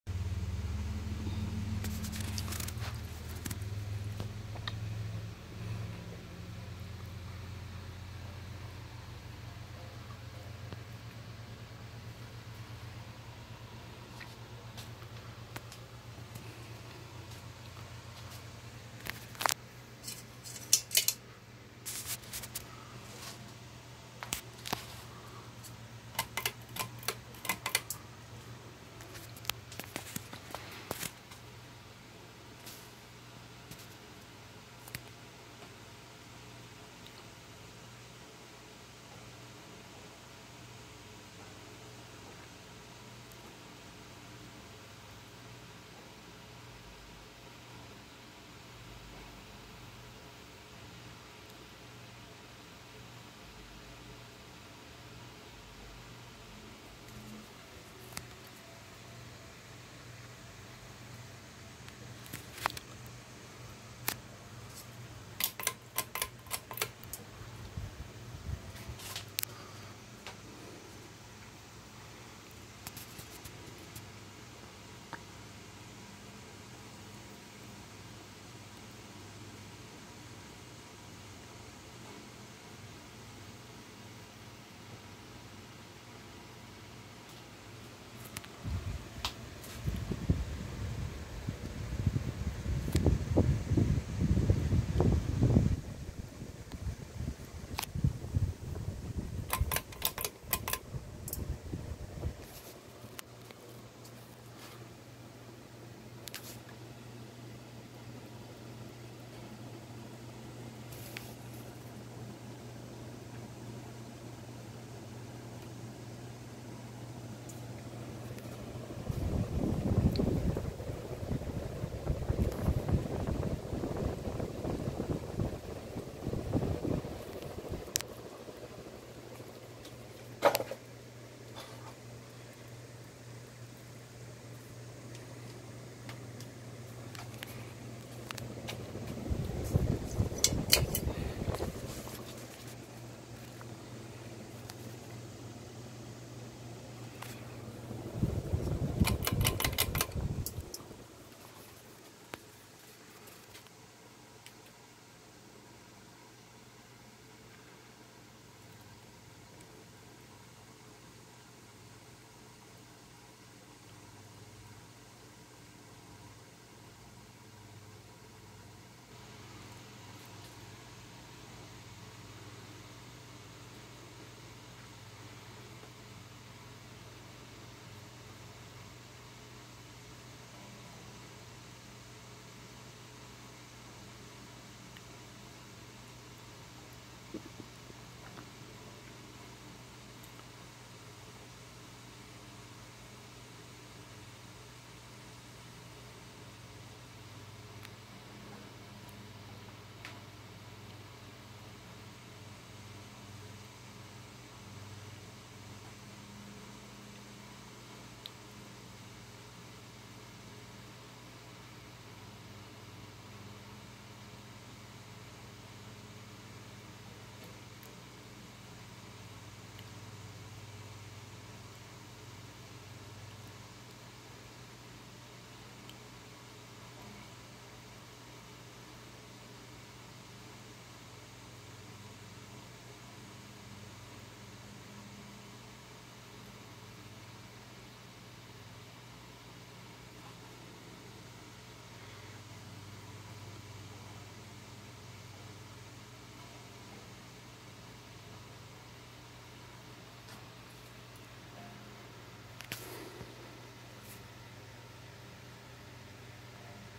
1986 SMC "The Emperor Flush" UT52 52" ceiling fan
Another fanimation find. reverse doesn't work on this one. I gotta put another reverse swich in it.